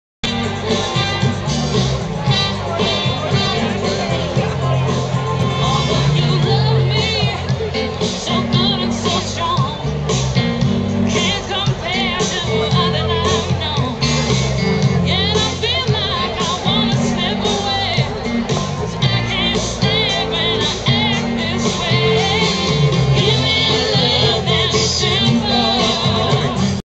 Illuminaqua '09
Concerts held on  a collapsable stage on the Canal. This is in between sets to show the stage
and the lighted fires